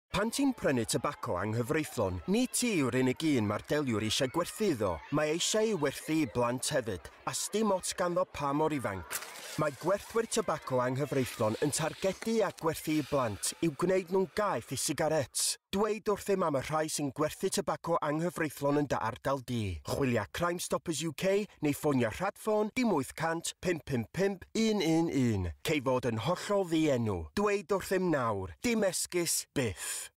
Dim esgus. Byth - Ymgyrch Tybaco Anghyfreithlon Cymru
Mae gwerthu tybaco anghyfreithlon yn broblem fawr yng Nghymru. Mae’n rhoi sigaréts yn nwylo plant, yn niweidio iechyd y cyhoedd ac yn gallu cyfrannu at droseddu cyfundrefnol difrifol. Os byddi di’n ei weld, RHO WYBOD amdano’n ddienw yn